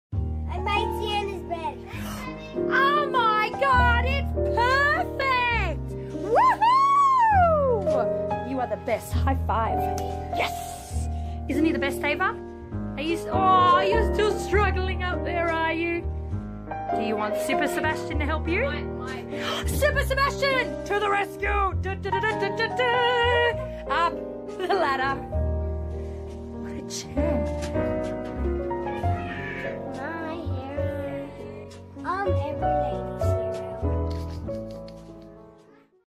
He helped me do what?! My hero!
As you all know I have special hands and arms and doing everyday things can sometimes be hard. today my best little buddy Sebastian helped me out! Check out what he did!